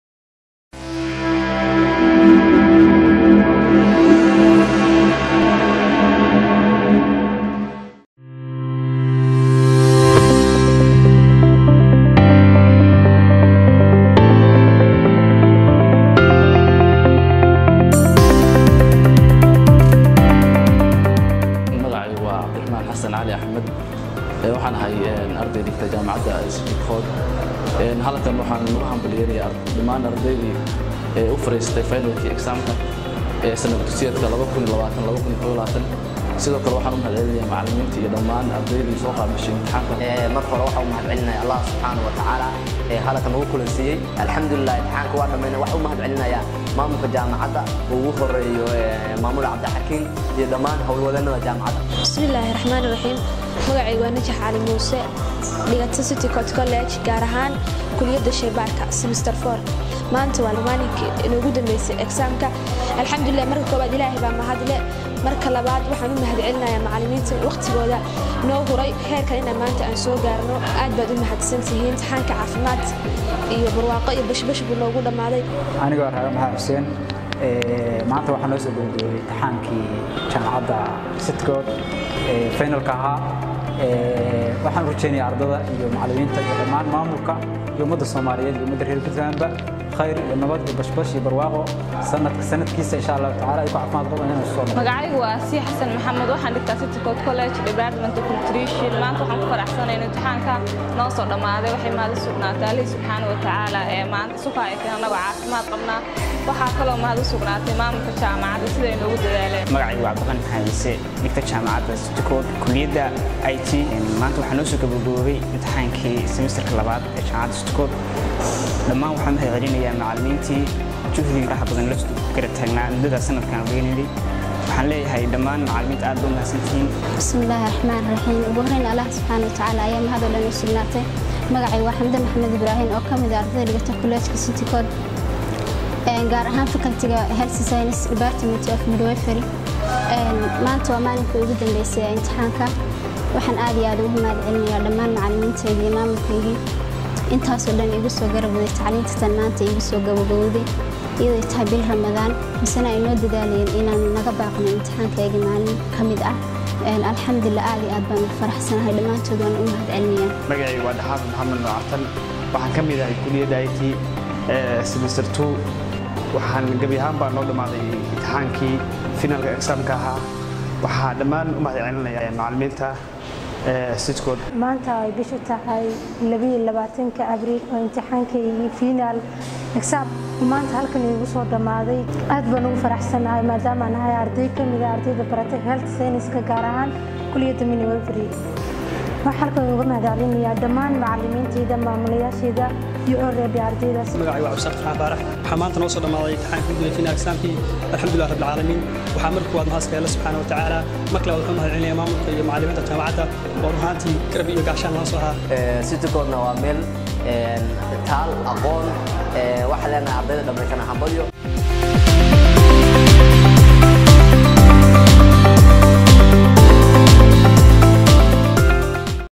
22 April dhamaadka imtixaanka jaamacadda sanad waxbaraheedka 2020-21
Ardyada waxay cabirayaan dareenkooda ku aaddan imtixaanka iyo mahadcelin ay u jeedinayaan jaamacadda, macalimiintooda iyo ardaydii ay muddada wax soo wada baranayeen.